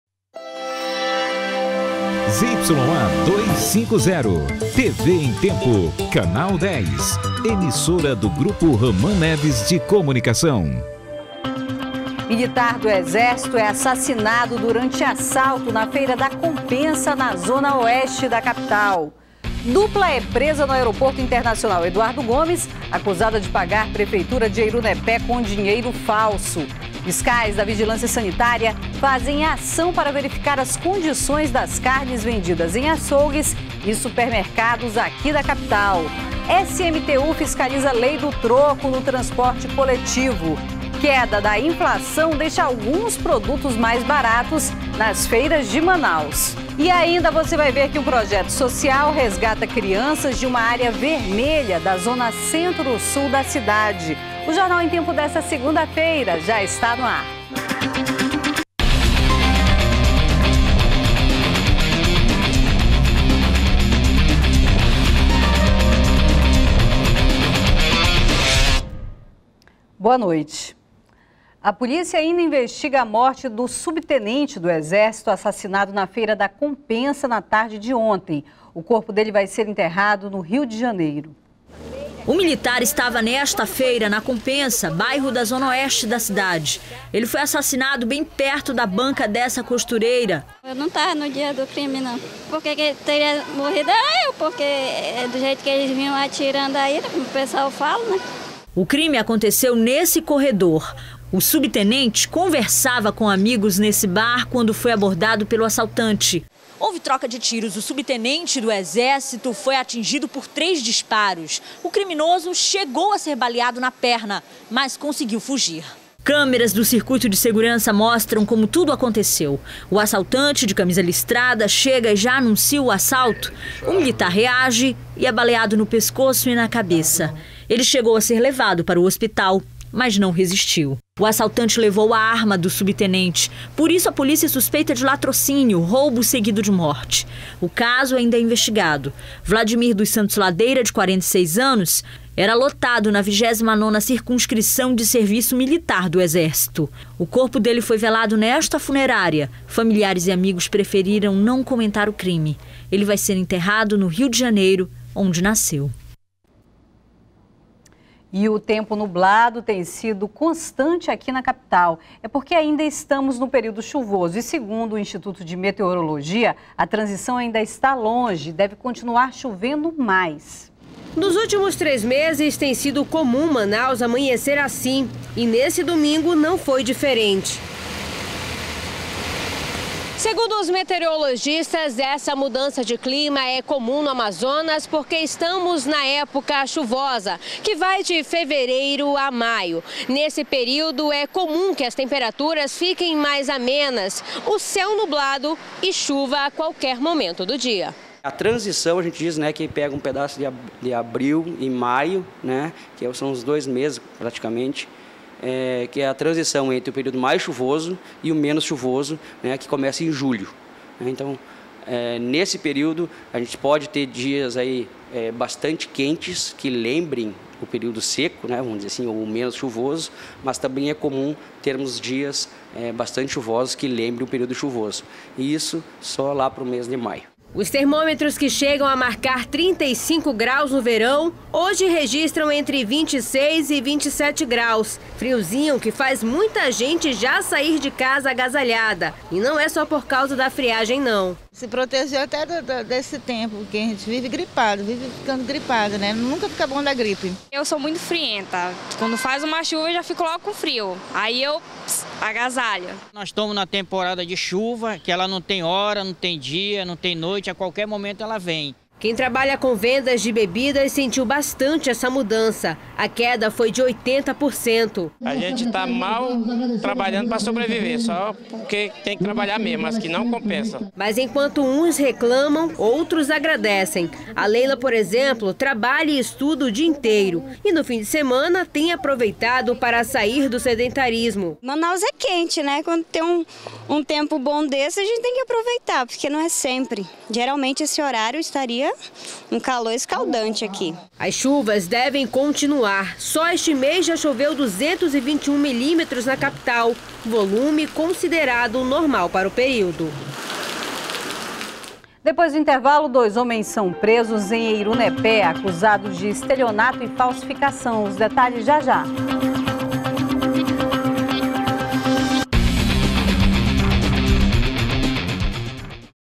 Militar do Exército é assassinado durante assalto na Feira da Compensa
01º BLOCO JORNAL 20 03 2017 - Militar do Exército é assassinado durante assalto na Feira da Compensa